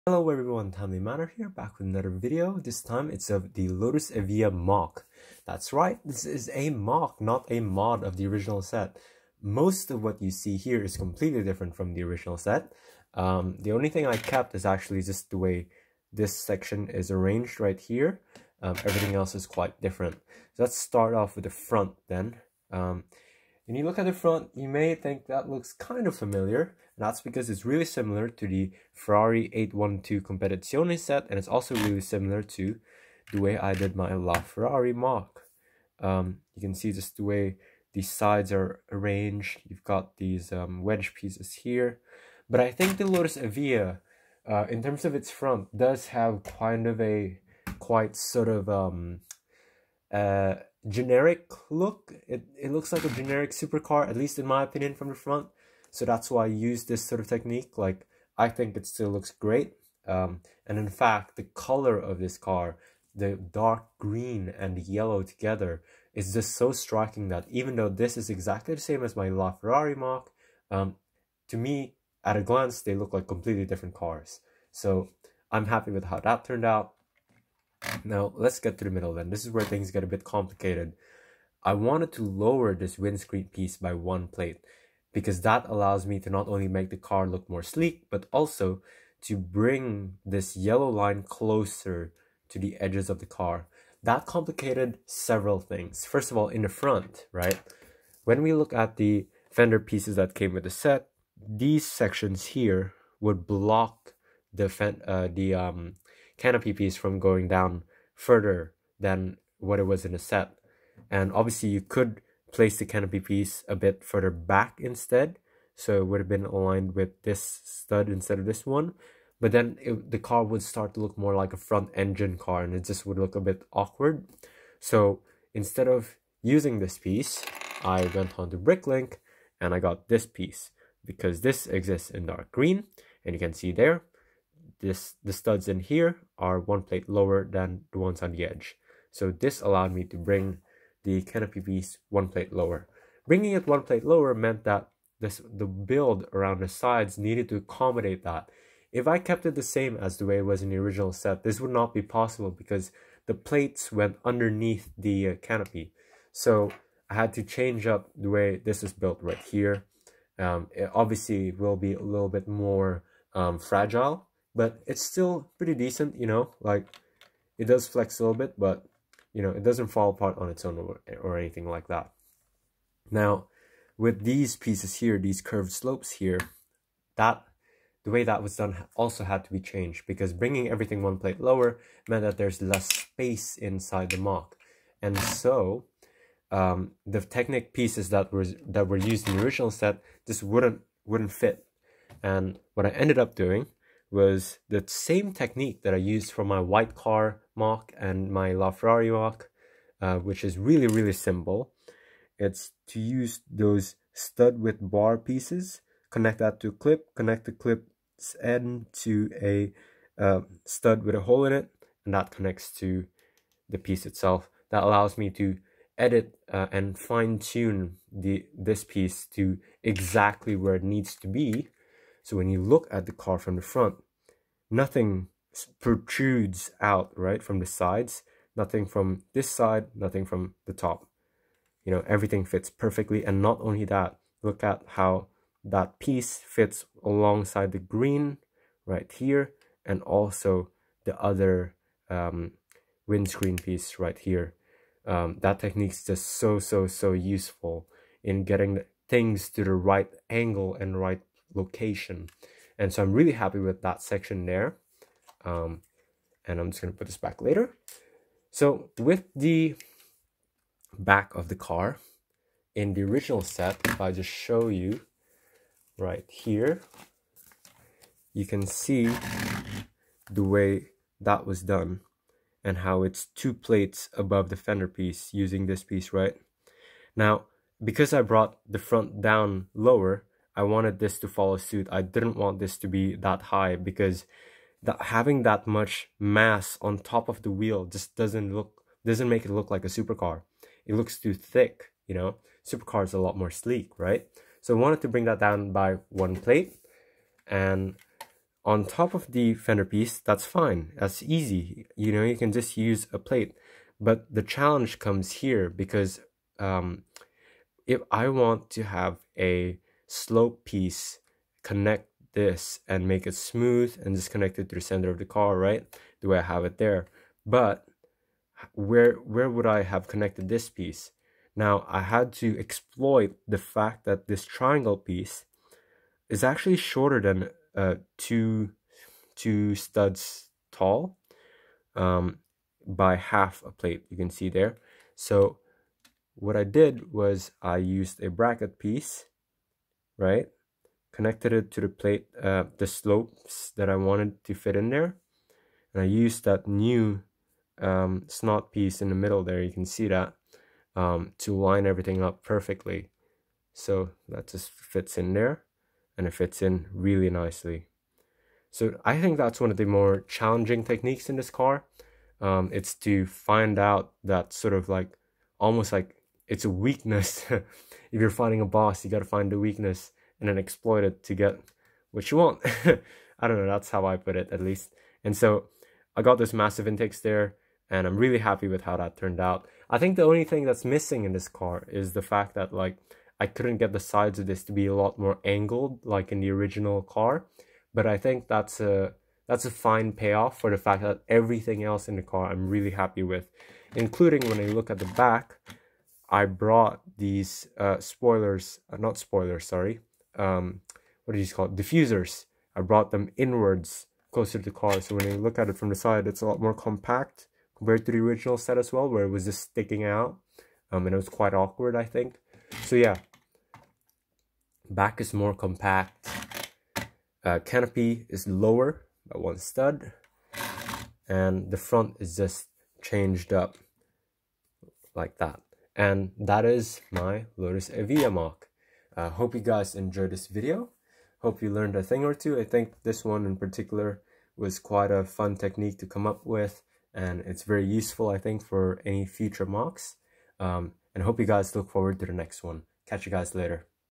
LEGO Lotus Evija MOC: Windscreen Problem Fix? Major Overhaul
I did a major redesign of the Lotus Evija set to bring the windscreen lower in an attempt to "fix" the horrible yellow line misprint on the windscreen.